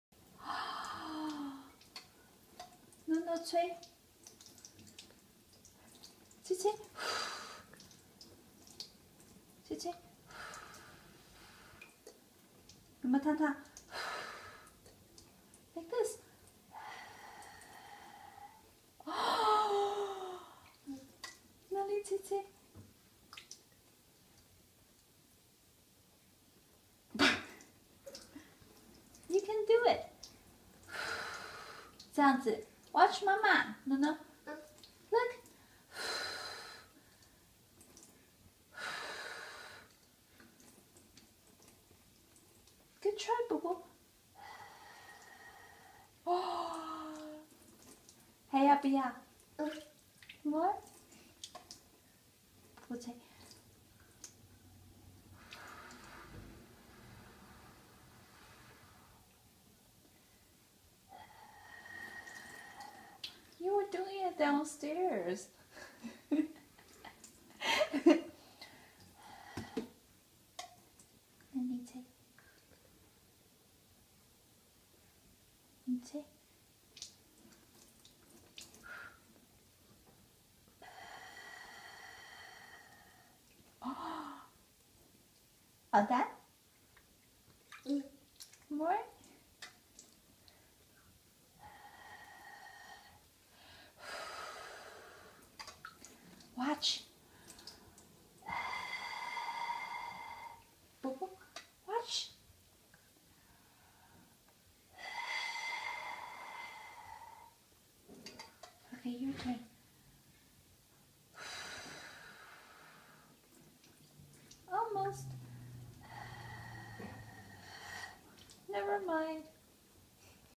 Baby Bubble Bath
Baby boy playing with bubbles during tub time.